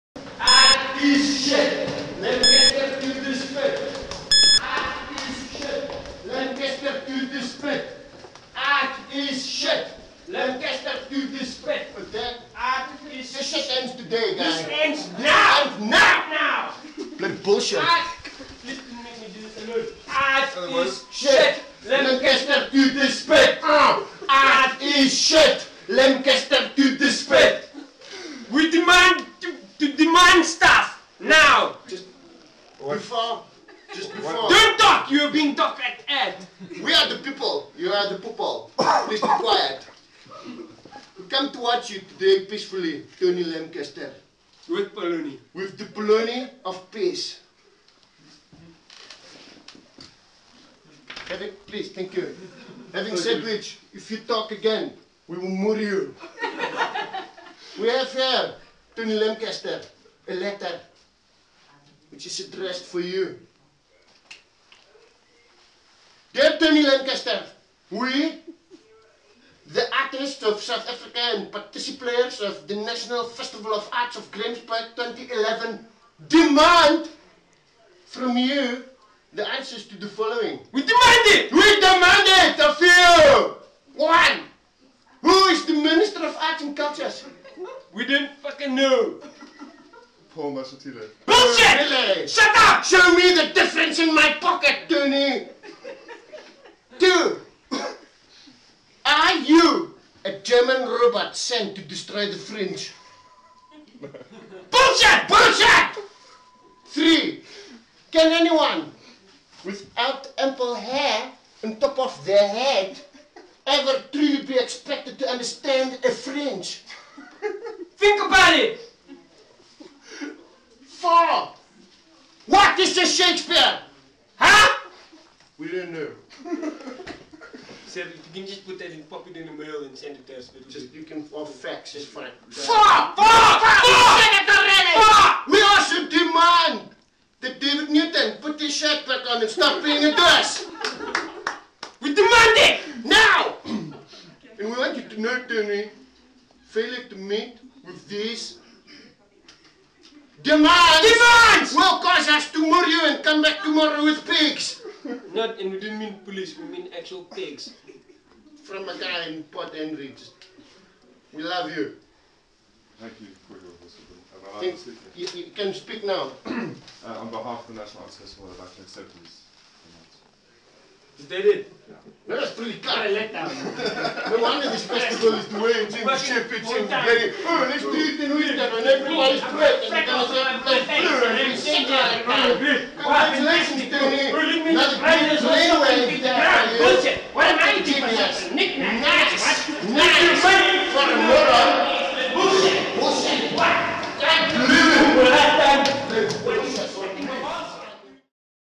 'Polony of Peace' for Fest CEO
Cultural terrorists, Corne and Twakkie (TMAS) storm National Arts Festival CEO, Tony Lankester's office with the Polony of Peace and to demand Dave Newton put his shirt back on (among other things). Jane Rosen & Jane van Doorene capture the brouhaha.